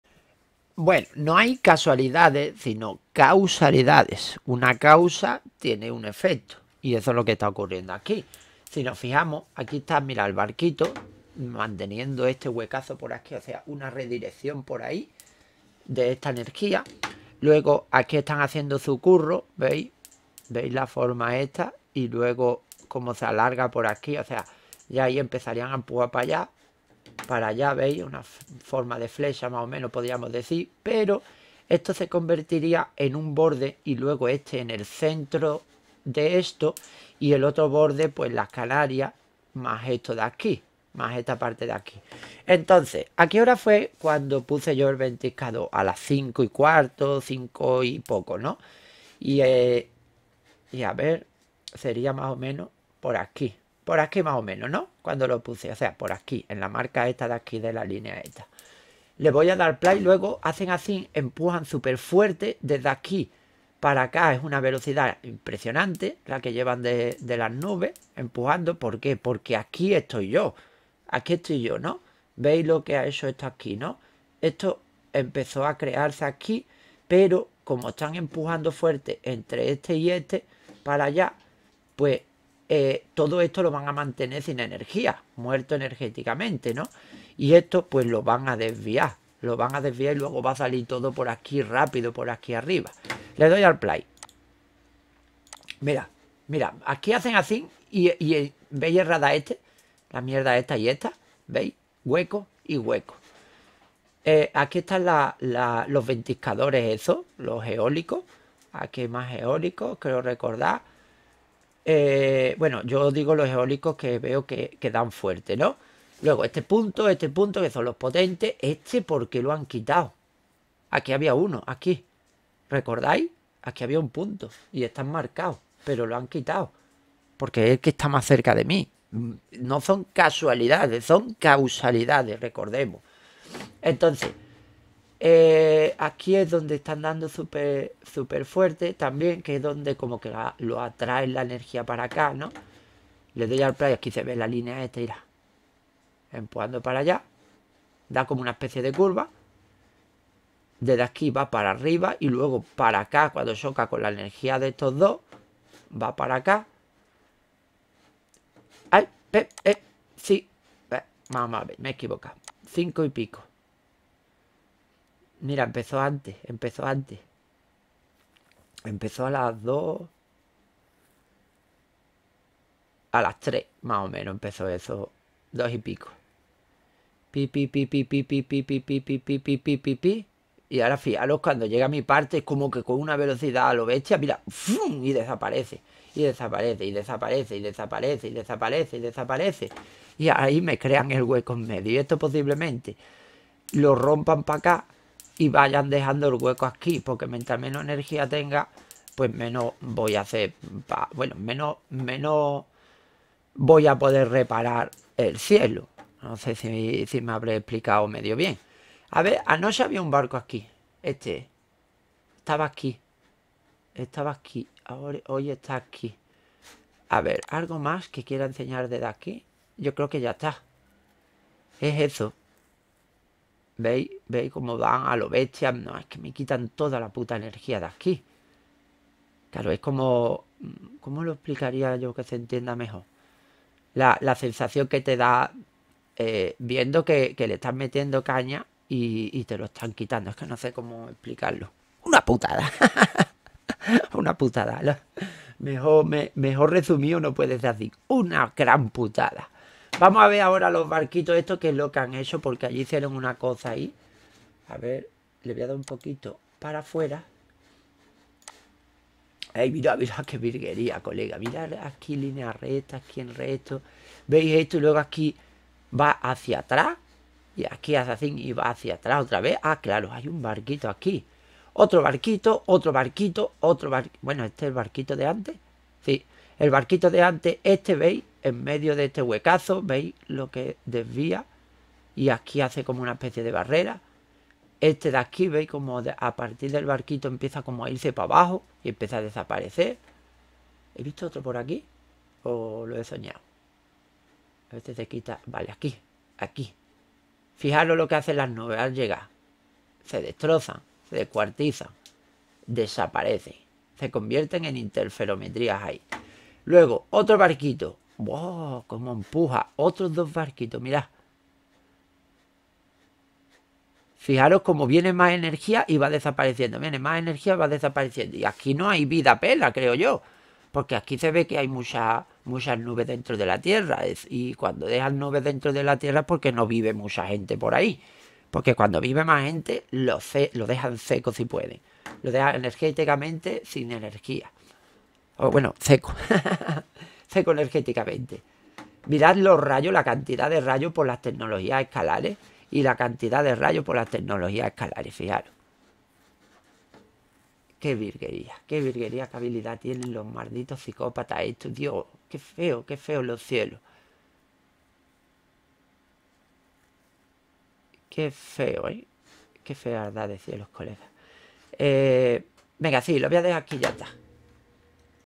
"2024-09-07" Las Causalidades
Este lo capture unas horas antes del ultimo clip, ahi explico más o menos como van empujando, le pongo contexto al clip anterior.